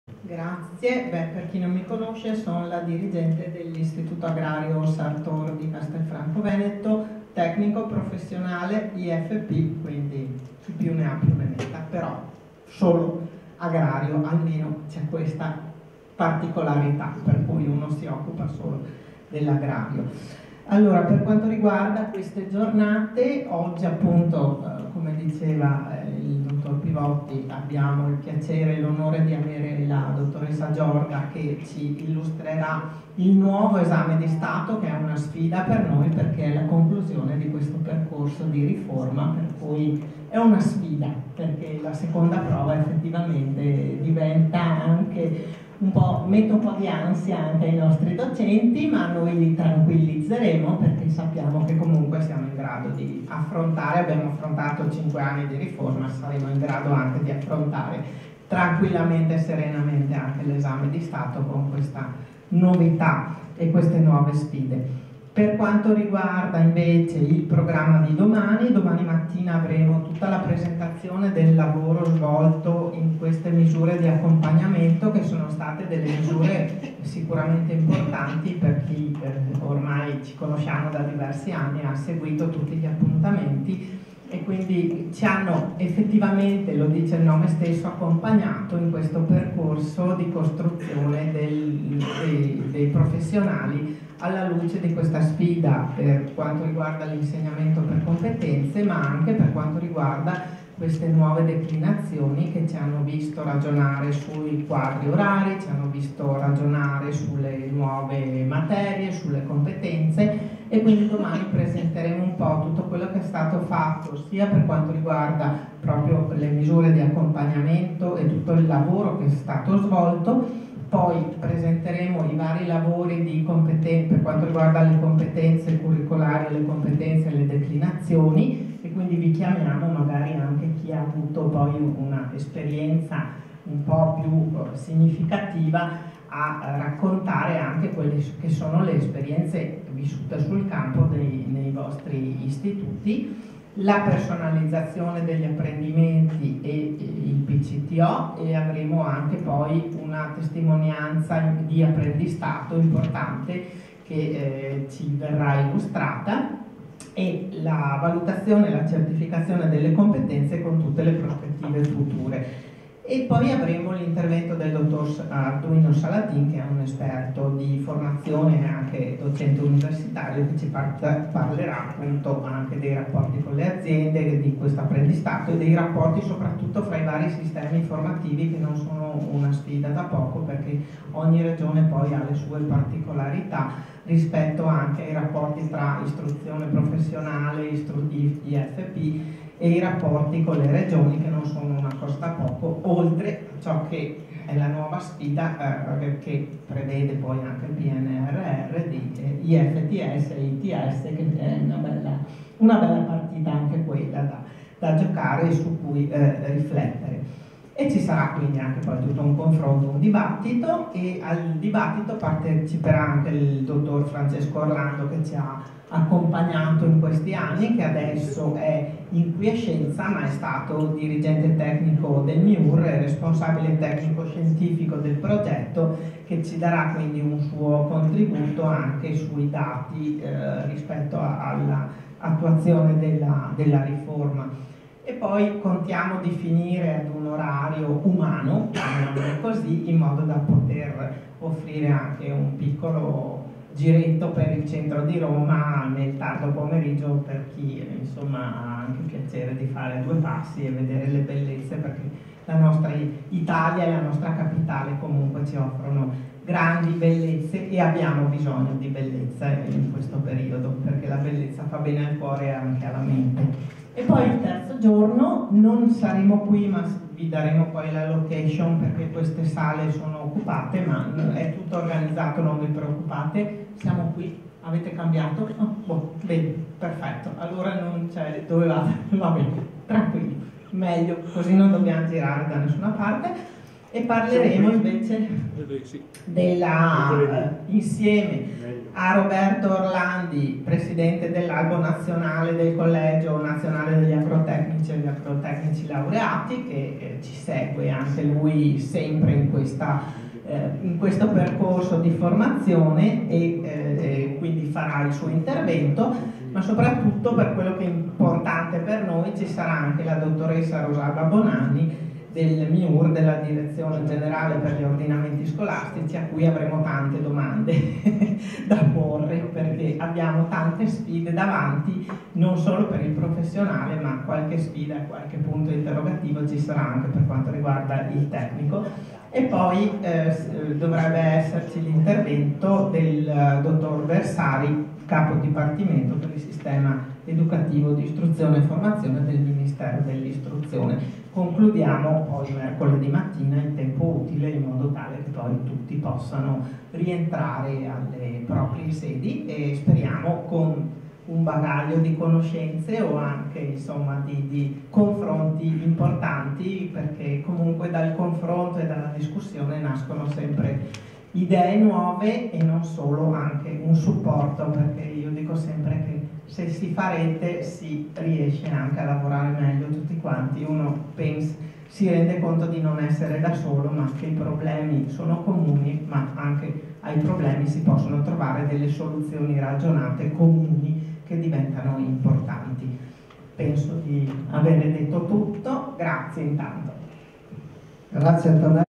Prof.ssa Alban - ROMA 17/10/22 Apertura, presentazione del Programma Mis.Acc.Rif.
ROMA 17/10/2022 -  Intervento di apertura della Prof.ssa Antonella Alban che presenta il programma alle scuole partecipanti al Convegno finale del Progetto “Gestione della innovazione e del miglioramento continuo nell’Istruzione professionale agraria”
Misure di supporto per l’attuazione del decreto legislativo 13
aprile 2017 n.61 Articolo 31-D.M. 851 del 27 ottobre 2017 per la
riforma degli Istruzione Professionale Agraria.